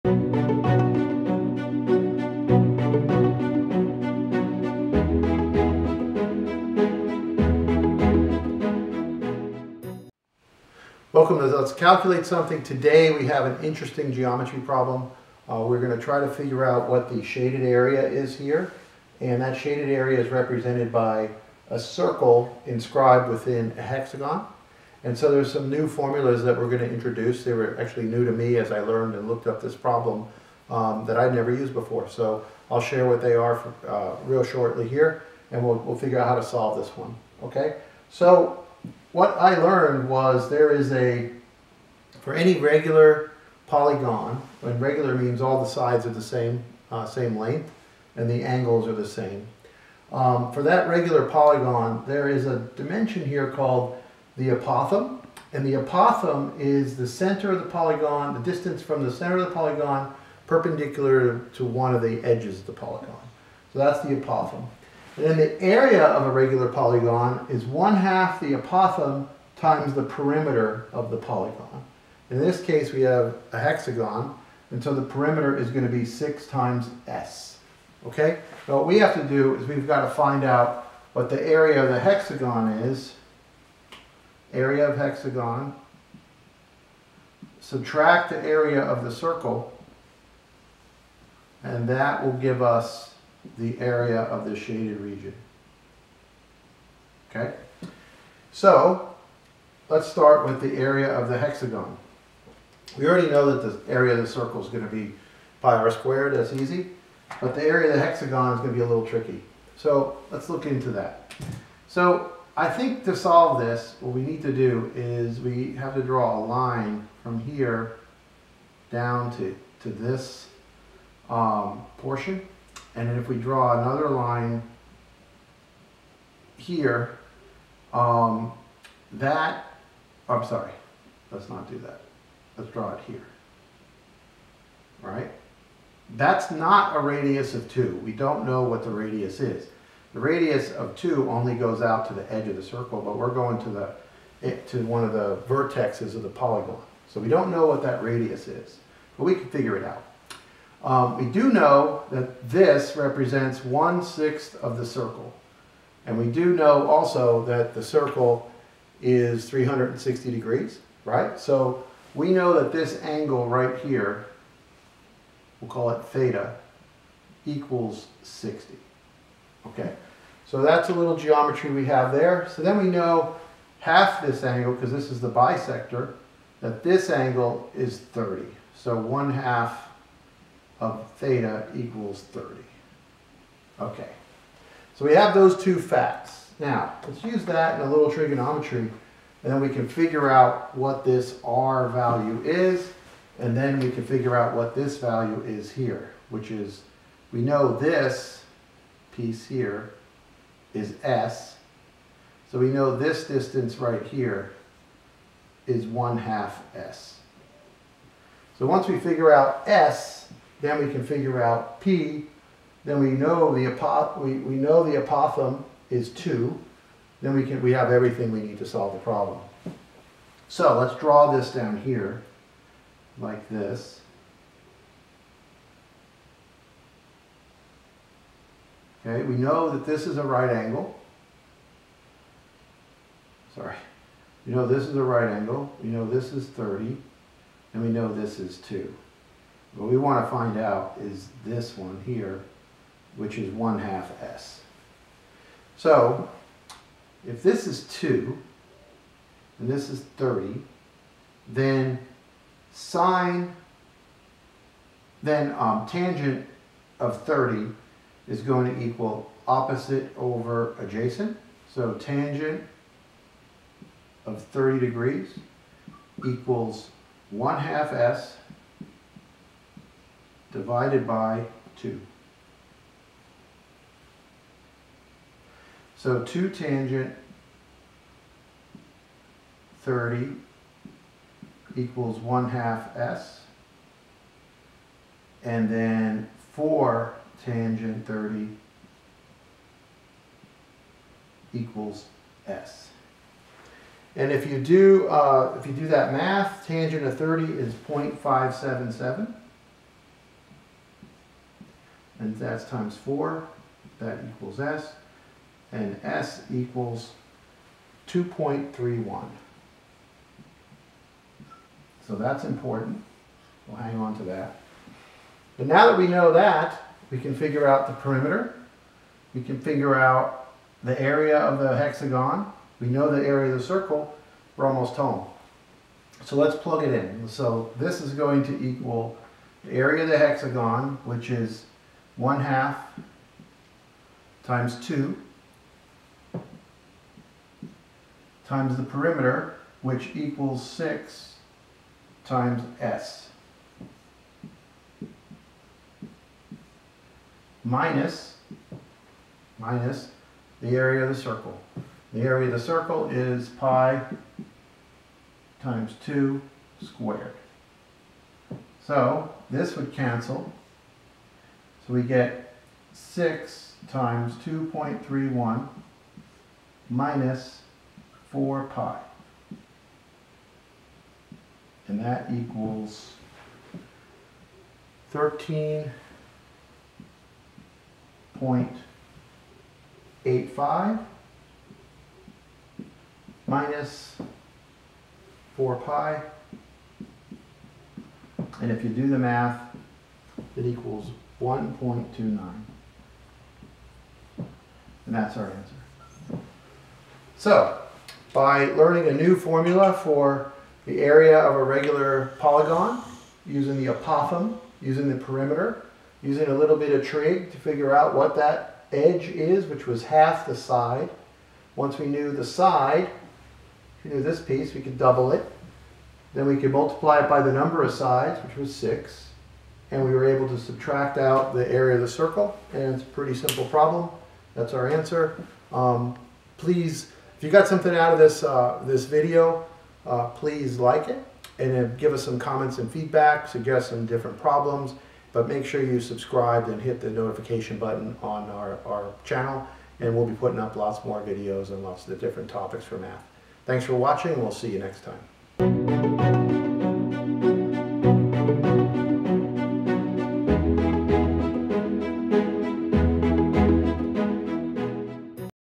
Can you calculate the shaded region? Circle inscribed in hexagon.
Video explaining the use of geometry to solve an are problem.  The video covers the solution with a clear explanation.  Uses geometry, paolygon and circle areas.

0:00 start
0:12 Explanation
0:45 polygon area explanation
0:55 apothem
1:45 Solve for area of hexagon
6:00 Solve for the area of the circle
6:30 answer
6:40 wrap-up

word problem
surface area
area of circle
circumference of circle
math functions
SAT Preparation
AP calculus
math Olympiad